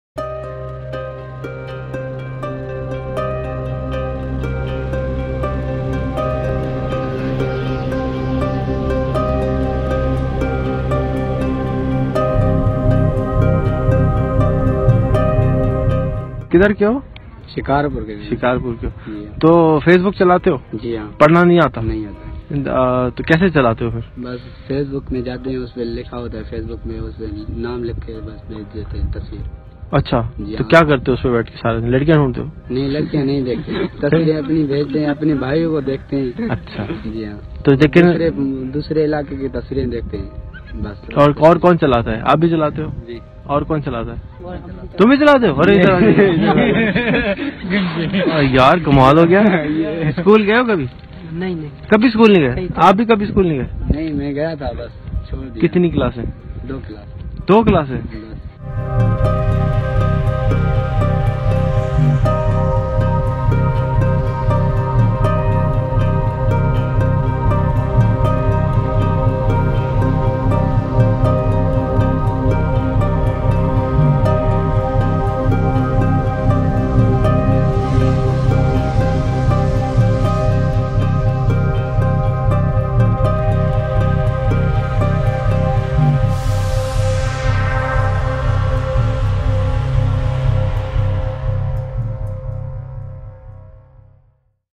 Facebook For Education | This Is Also Pakistan | Rehan Allahwala
While I was in Gwadar, Balochistan, Pakistan, I met a team of 4 men who were daily wagers, I was amazed to see them using Facebook. They were using Facebook to get connected with their loved one and get updated to from what was happening around them. I was not expecting to meet someone in Gwadar was using Facebook and was happy to them using Facebook.